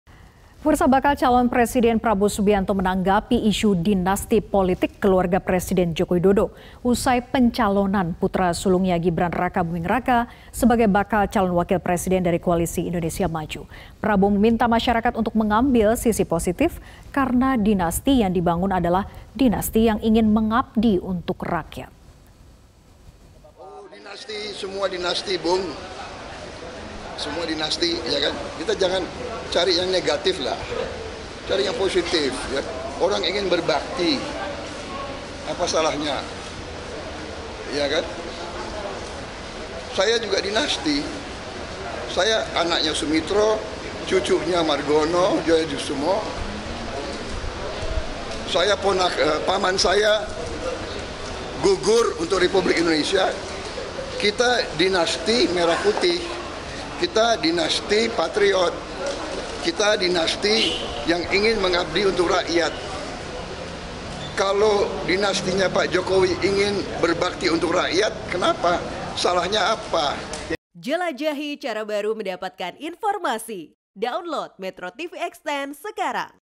Prabowo: Kalau Dinasti Jokowi Berbakti untuk Rakyat, Salahnya Apa?
MetroTV, : Ketua Umum Partai Gerindra Prabowo Subianto menyampaikan pembelaan soal anggapan politik dinasti usai menggandeng Wali Kota Solo Gibran Rakabuming Raka. Gibran merupakan anak sulung Presiden Joko Widodo (Jokowi).

Menteri Pertahanan itu klaim merupakan dinasti merah putih dan dinasti patriot. Prabowo menyebut senada dengan Jokowi yang menerapkan dinasti politik berbakti kepada rakyat.

"Kalau dinastinya Pak Jokowi ini berbakti untuk rakyat, kenapa? Salahnya apa? Jadi berpikir yang baiklah. Berpikir positif, ya," ujar Prabowo.